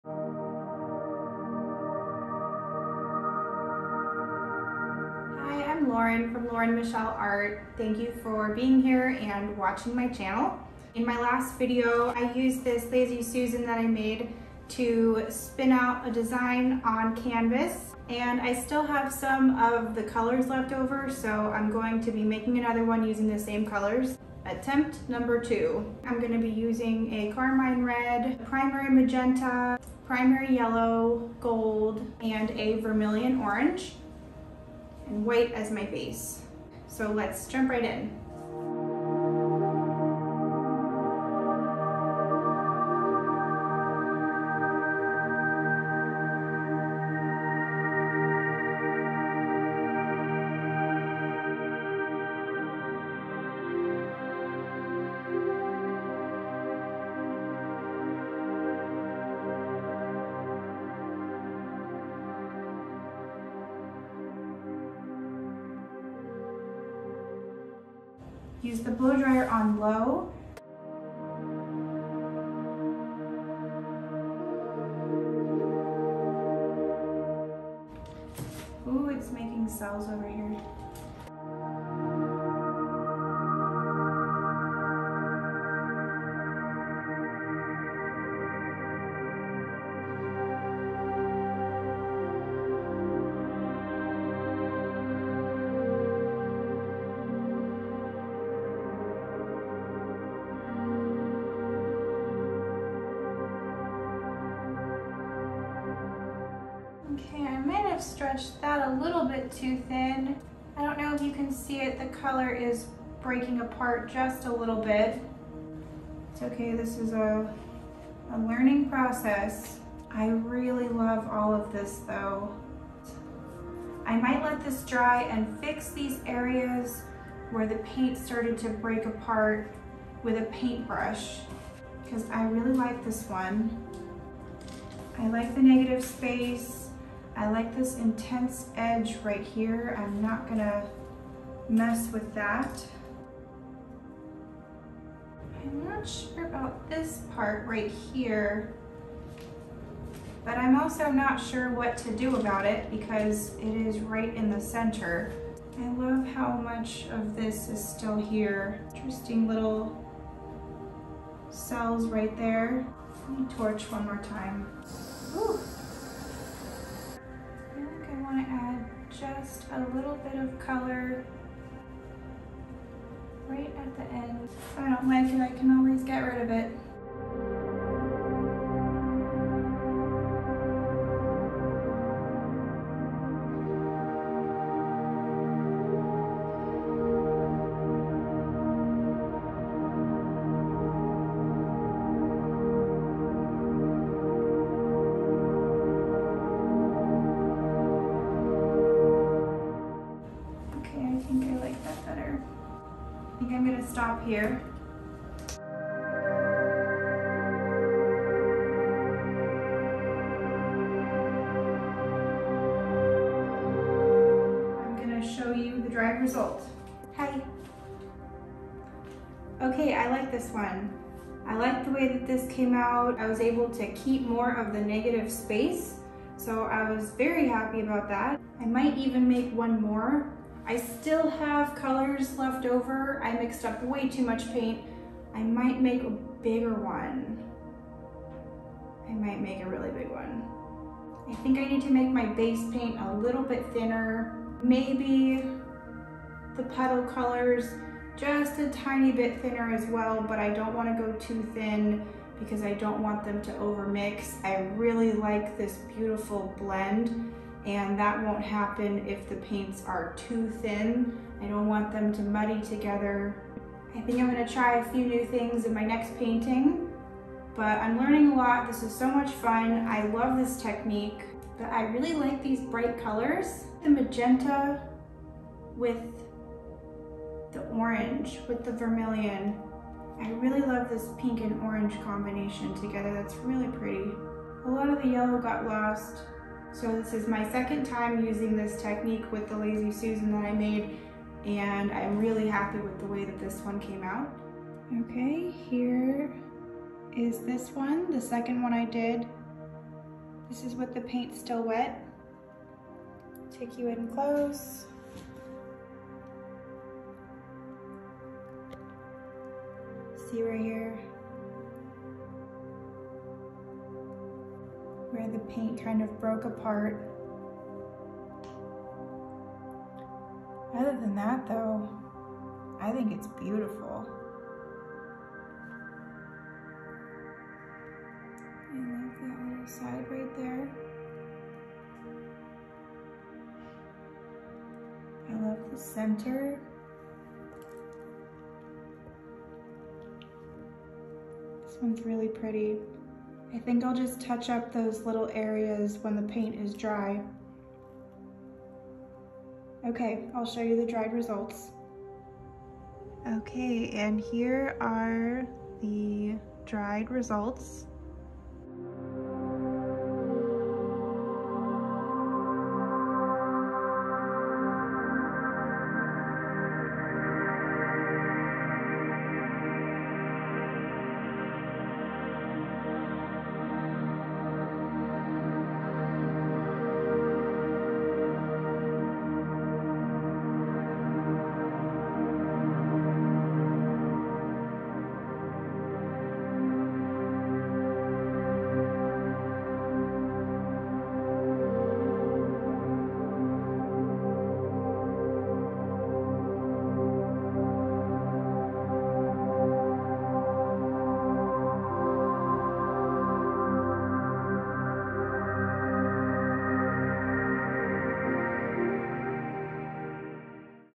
STARBURST Dutch Pour Painting on the Turntable - Pink and Orange | Acrylic Pouring | Fluid Art
Hi everyone, this was my second time using the lazy susan turntable I built to create a Dutch pour acrylic painting. I used red, magenta, orange and yellow, a gold center and a white base. For the Amsterdam paint I used just paint and water and for the Soho gold I added a small amount of creative inspirations gloss medium and varnish. This style of artwork is so unique and created such a beautiful effect. I’ll definitely be doing more of these!

All of my artwork is for sale.

Amazon Storefront